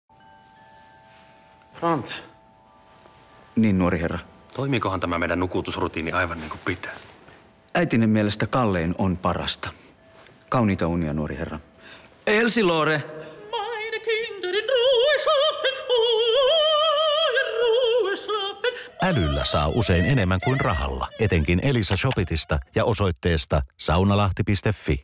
Tuutulaulu - Rikkaat ja Järjettömät - Saunalahden TV-mainos
Hehkeä Elsilore serenoi tuutulaulun epäileväiselle nuori herra Herbertille. Butler muistuttaa, että äidin mielestä kallein on parasta.

Älyllä saa usein enemmän, kuin rahalla. Lisätietoa Elisa Shopitista tai osoitteesta